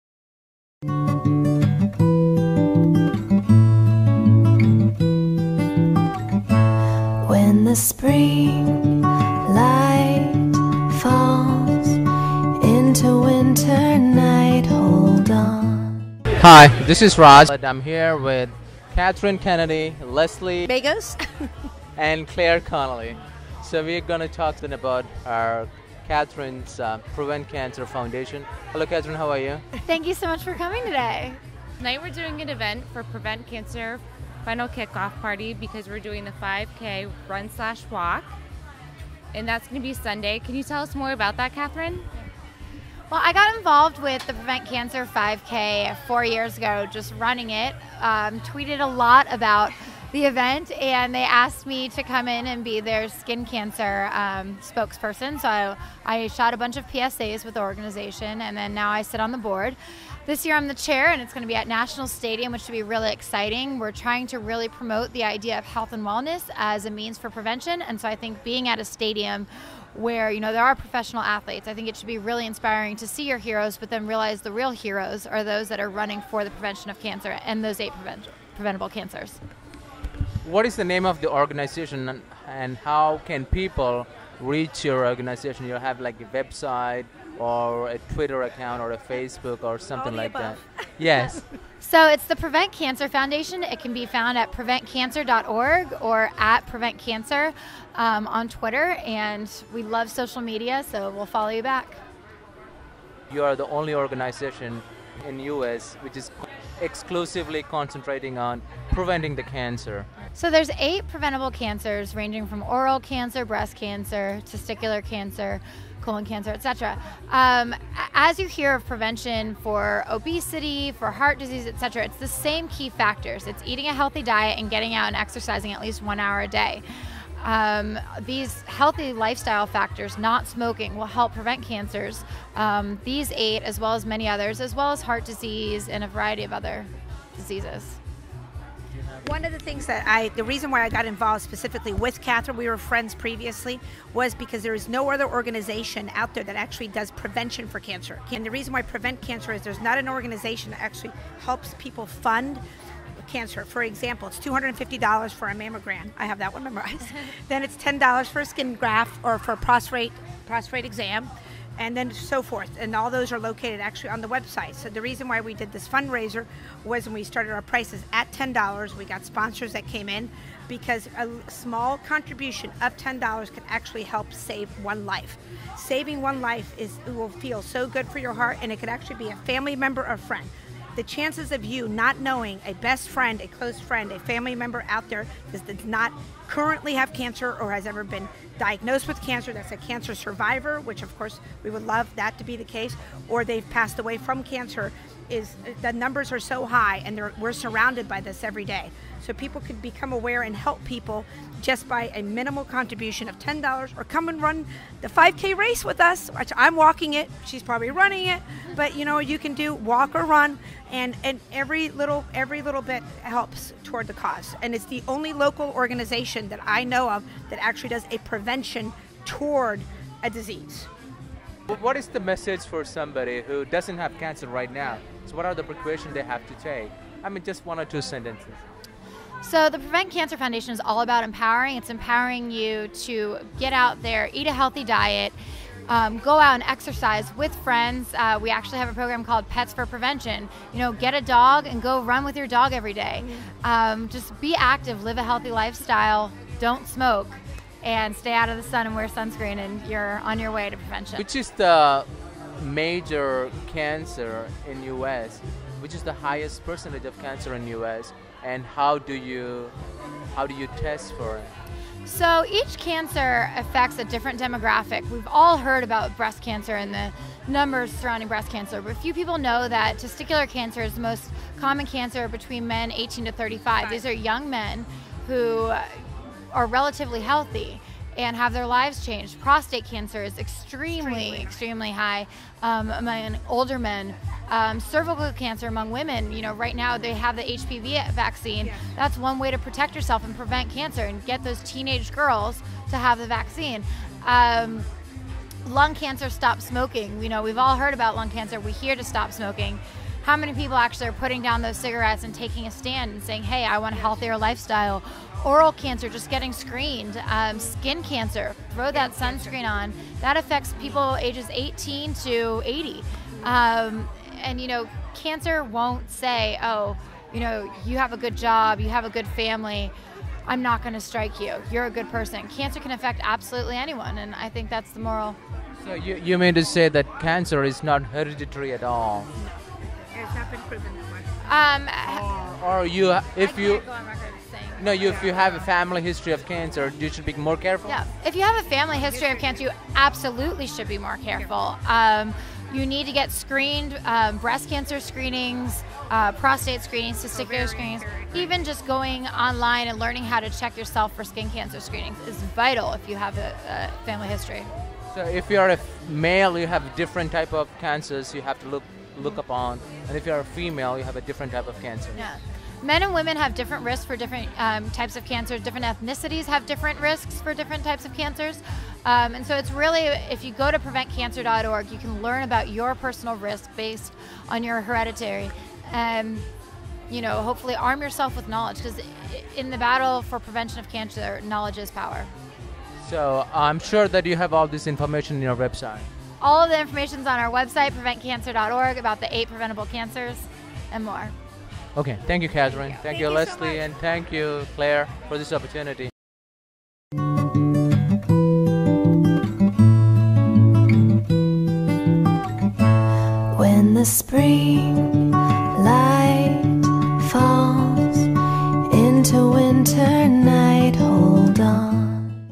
Interview with Katherine Kennedy for Prevent Cancer Foundation
Trying to raise awareness for prevent cancer in men and women. There are 8 types of cancers that can be preventable. Katherine Kennedy explains it all in the video

and read the description there.